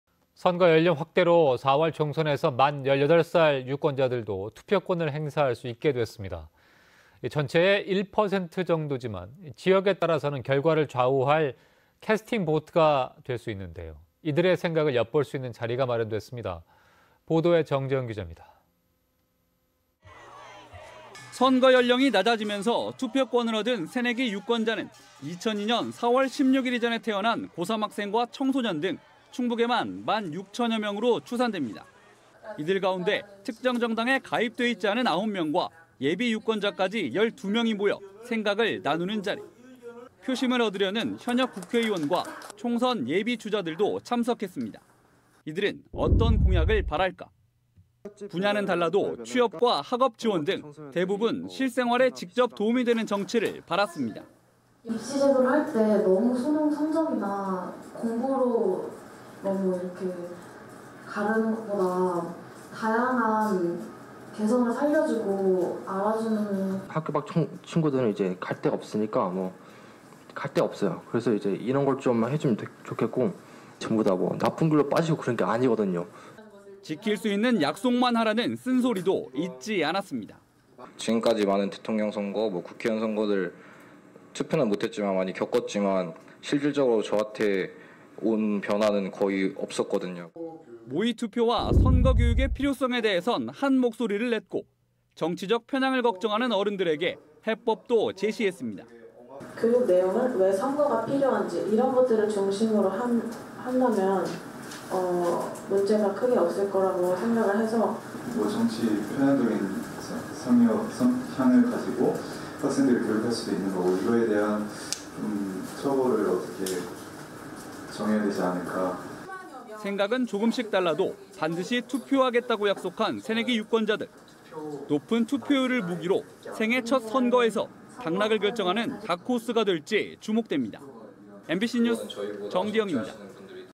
'1%의 힘' 새내기 유권자 어디로?ㅣMBC충북NEWS
[앵커]
 선거 연령 확대로 4월 총선에서
만 18살 유권자들도 투표권을
행사할 수 있게 됐습니다.

 전체의 1% 정도지만 지역에 따라서는
결과를 좌우할 캐스팅보트가 될 수 있는데요.
이들의 생각을 엿볼 수 있는 자리가
마련됐습니다. 정재영 기자입니다.

    [기자]
 선거 연령이 낮아지면서
투표권을 얻은 새내기 유권자는
2002년 4월 16일 이전에 태어난 고3 학생과
청소년 등 충북에만 1만 6천여 명으로
추산됩니다.

 이들 가운데
특정 정당에 가입돼 있지 않은 9명과
예비유권자까지 12명이 모여
생각을 나누는 자리.

 표심을 얻으려는 현역 국회의원과
총선 예비주자들도 참석했습니다.

 이들은 어떤 공약을 바랄까?

  (CG) 분야는 달라도 취업과 학업 지원 등
대부분 실생활에 직접 도움이 되는 정치를
바랐습니다.

 [류호정/새내기 유권자]
"입시제도를 할 때 너무 수능 성적이나
공부로 가르는 것보다 다양한 개성을 살려주고,
알아주는."

 [이유성/새내기 유권자]
"학교 밖 청소년들은 갈 곳이 없어요. 이런 걸
지원해줬으면 좋겠고. (학교 나왔다고) 전부 다
나쁜 길로 빠지는 건 아니거든요."

 지킬 수 있는 약속만 하라는 쓴소리도
잊지 않았습니다.

 [박세진/새내기 유권자]
"지금까지 대통령 선거, 국회의원 선거들
투표는 못했지만 많이 겪었는데 실질적으로
저한테 온 변화는 거의 없었거든요."

(CG) 모의투표와 선거 교육의 필요성에 대해선
한 목소리를 냈고 정치적 편향을 걱정하는
어른들에게 해법도 제시했습니다.

 [김지담/새내기 유권자]